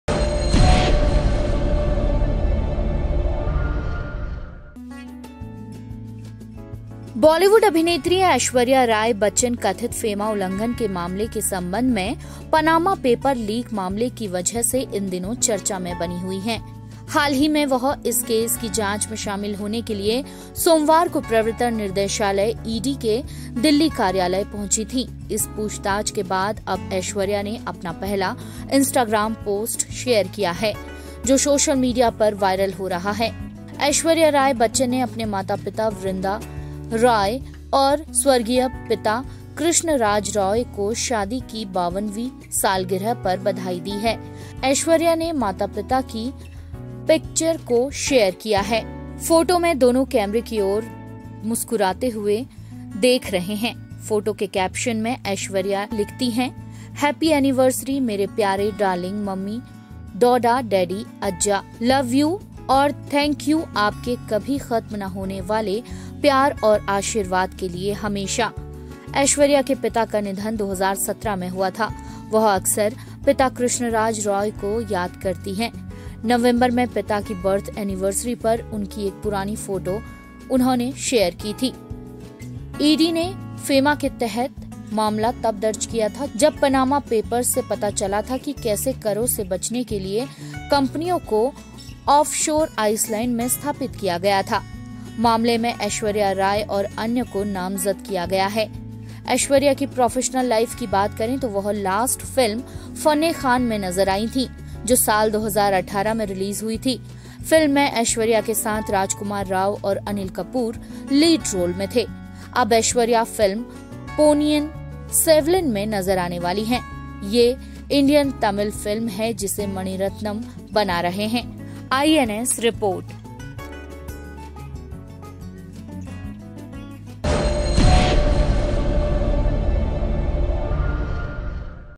ऐश्वर्या राय बच्चन ने पनामा पेपर्स मामले में पूछताछ के बाद शेयर किया पहला पोस्ट
बॉलीवुड अभिनेत्री ऐश्वर्या राय बच्चन कथित फेमा उल्लंघन के मामले के संबंध में पनामा पेपर लीक मामले की वजह से इन दिनों चर्चा में बनी हुई हैं। हाल ही में वह इस केस की जांच में शामिल होने के लिए सोमवार को प्रवर्तन निदेशालय ;ईडी  के दिल्ली कार्यालय पहुंचीं के थी ।  इस पूछताछ के बाद अब ऐश्वर्या ने अपना पहला इंस्टाग्राम पोस्ट शेयर किया है.